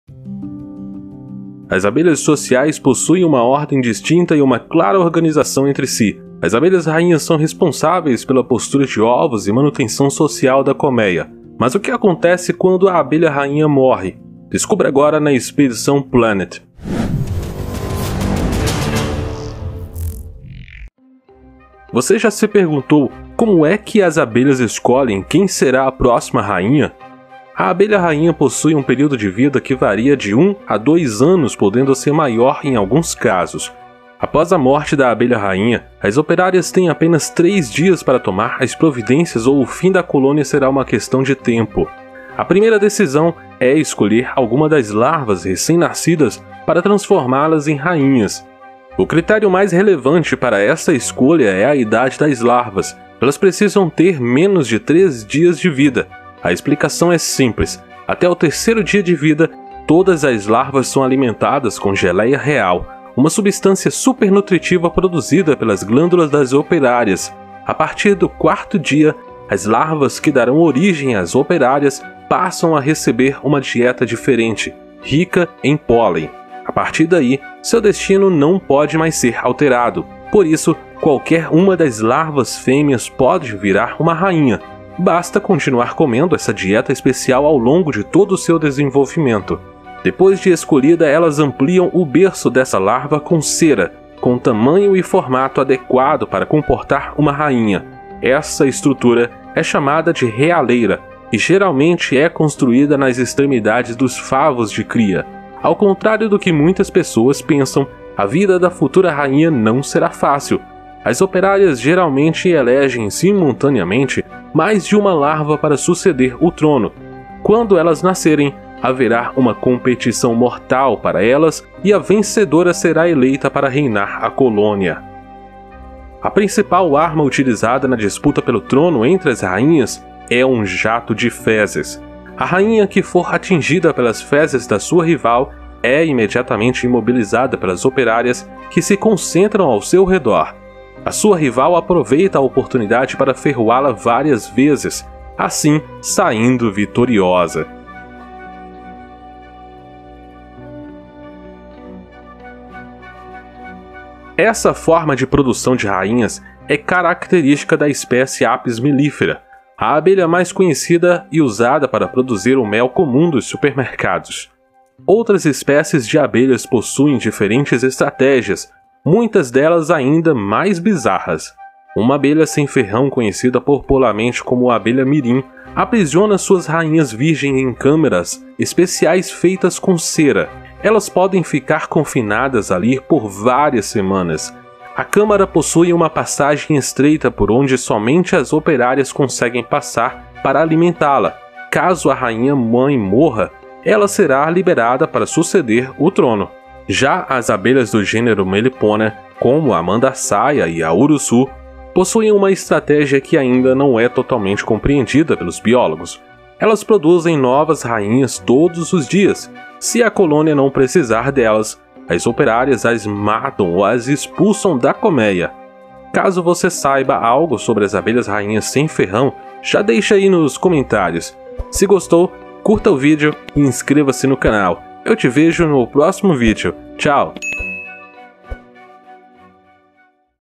Como as abelhas escolhem a próxima rainha
Você já se perguntou como é que as abelhas escolhem quem será a próxima rainha?
A abelha rainha possui um período de vida que varia de 1 a 2 anos podendo ser maior alguns casos, após a morte da abelha rainha as operárias têm apenas três dias para tomar as providências ou o fim da colônia será uma questão de tempo.